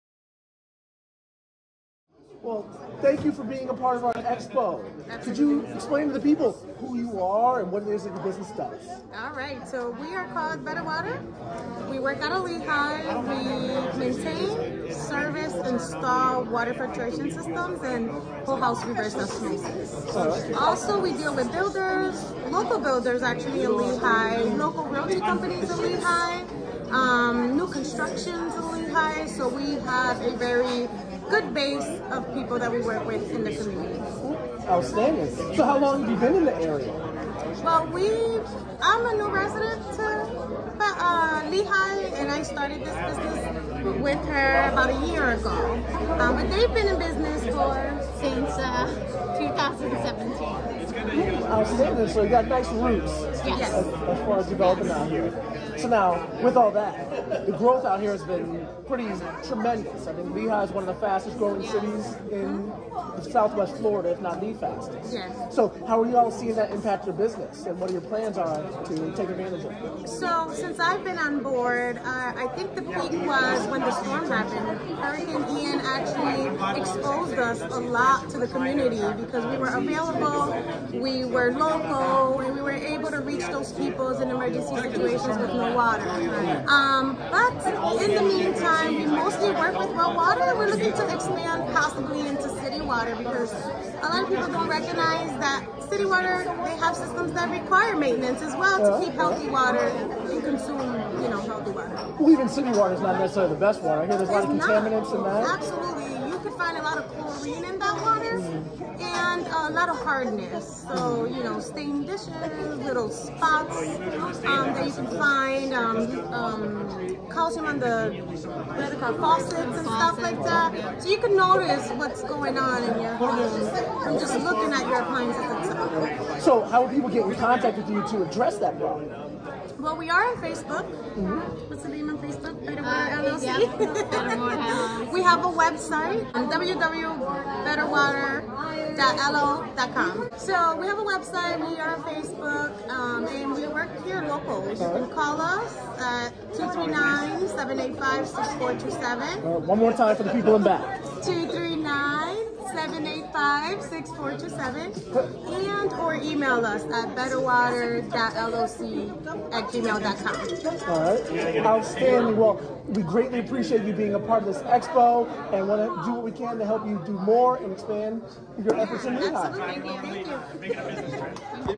Rotary-KOC Ladies Auxiliary Business Expo: Better Water
Rotary Club of Lehigh Acres and the Knights of Columbus Ladies Auxiliary 1727 host the Rotary Means Business Fellowship Expo

Better Water is locally owned serving the Lee County Collier County area. We use the highest quality equipment to fulfill all water needs.
water softener, water treatment, salt delivery, water systems, reverse osmosis.

Frank Gonzalez
Water Filtration & Purification Equipment
Open: 9:00 am - 5:00 pm M-F